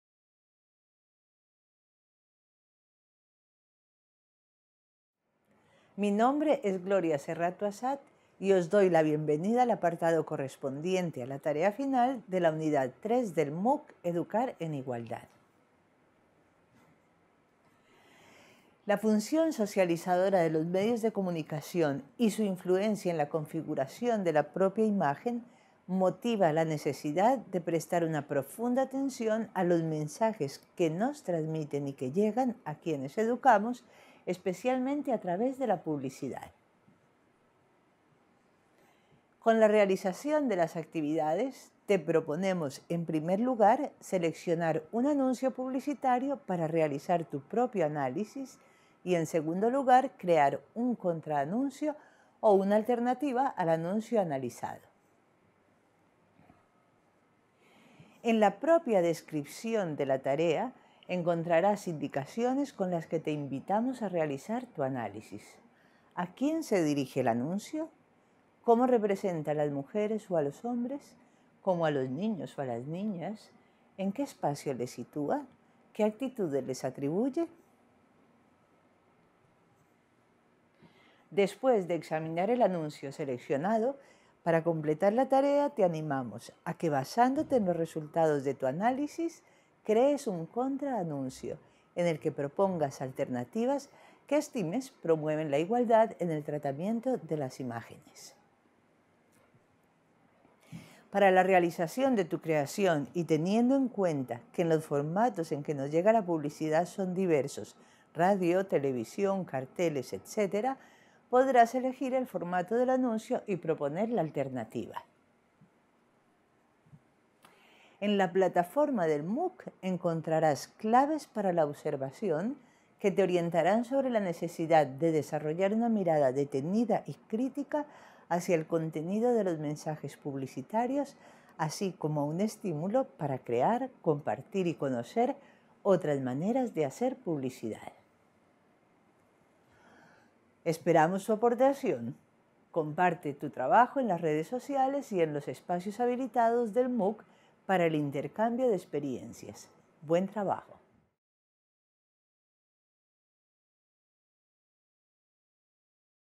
Vídeo 3.3. "La influencia de los medios de comunicación" - Reto
Vídeo 3 de la Unidad 3 del MOOC de AprendeINTEF "Educar en Igualdad" EducaIguales. Para más información, visita